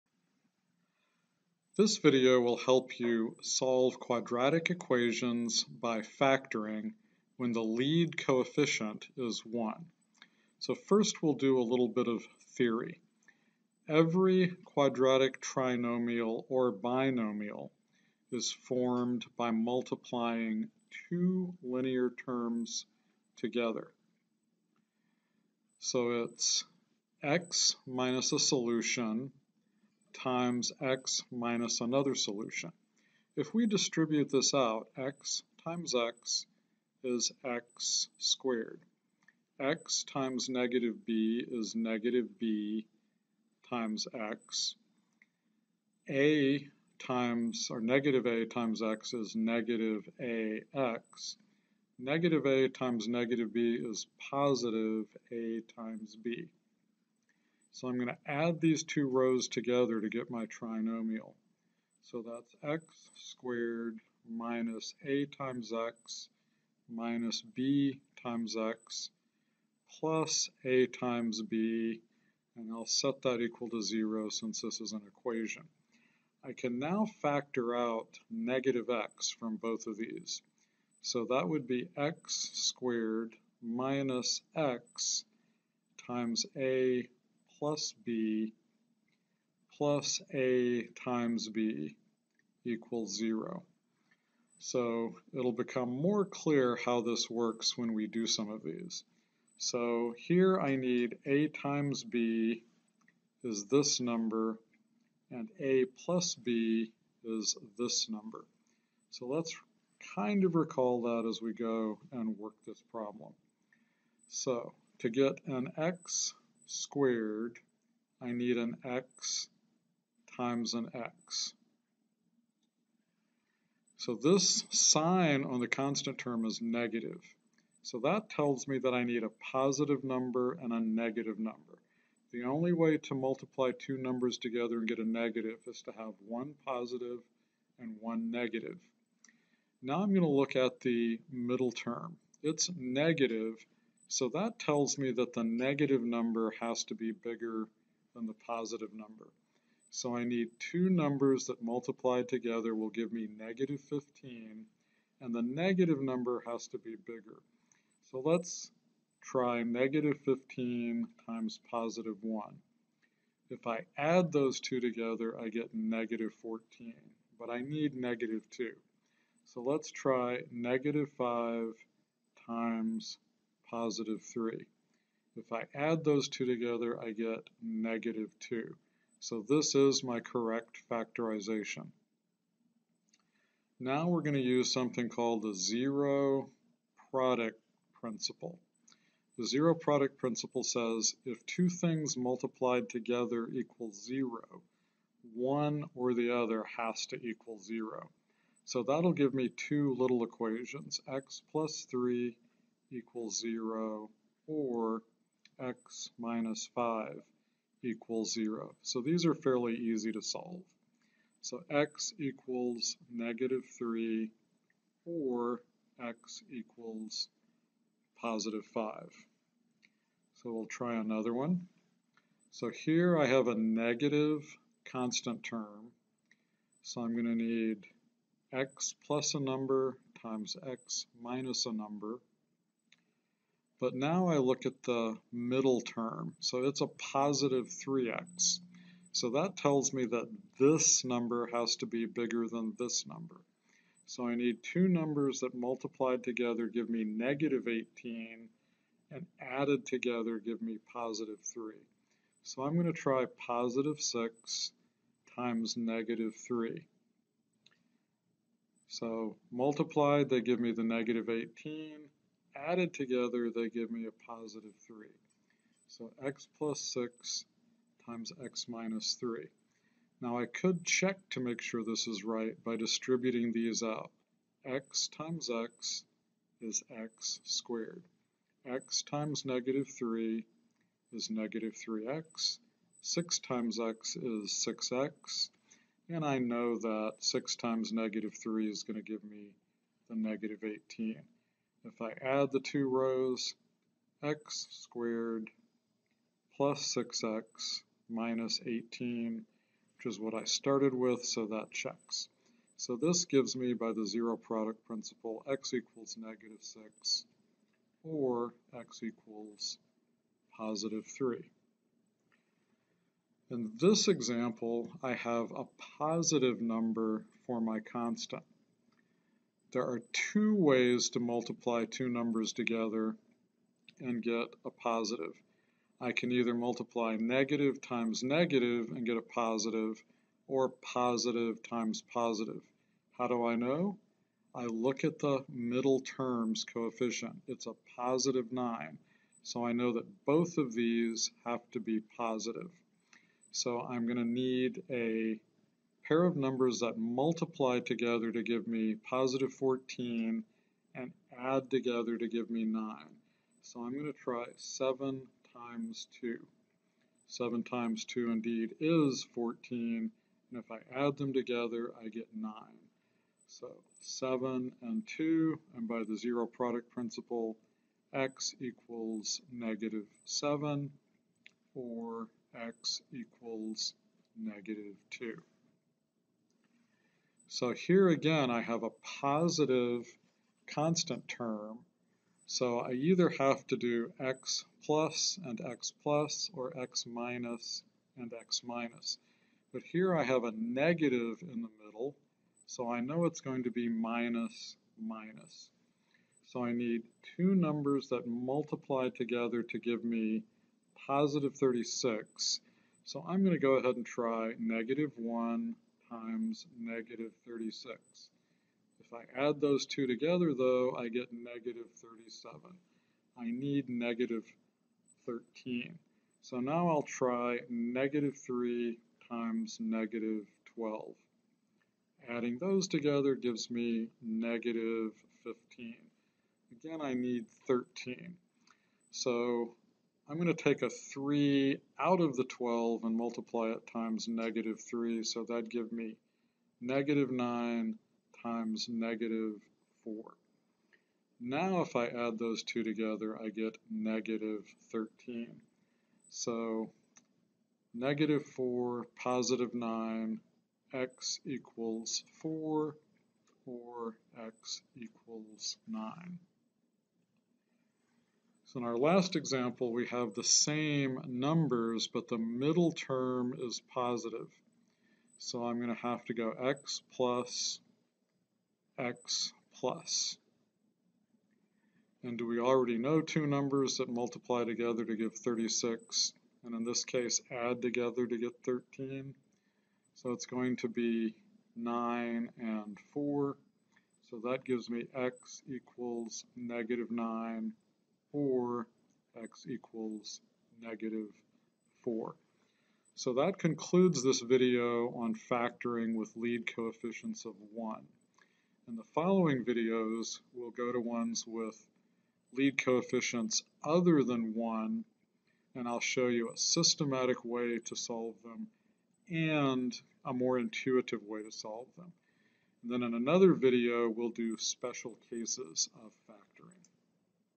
Factoring1
Solving quadratic equations by factoring with lead coefficients of one.